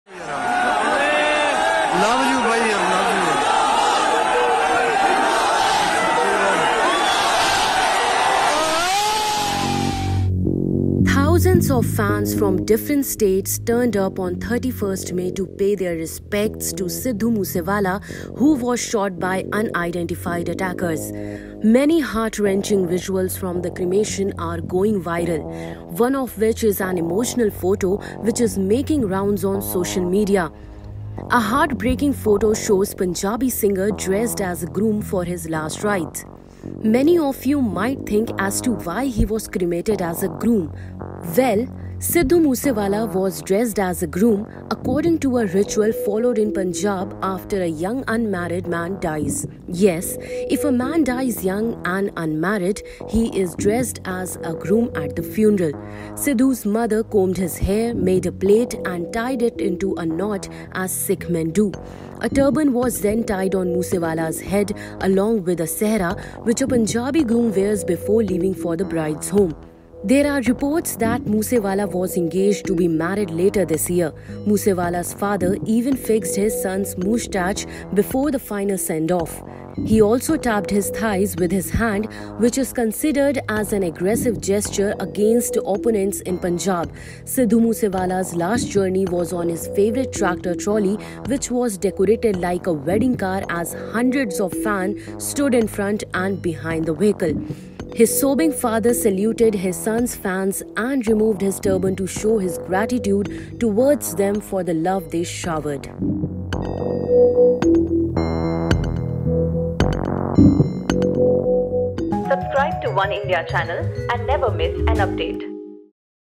Sidhu Moosewala was cremated as a Groom; Here's Why | Oneindia News
Sidhu Moosewala, who was shot dead by unidentified attackers, was cremated as a groom. Yes, a photo of Sidhu dressed as a groom for his last ride is making rounds on social media. Watch to know why he was cremated as a groom.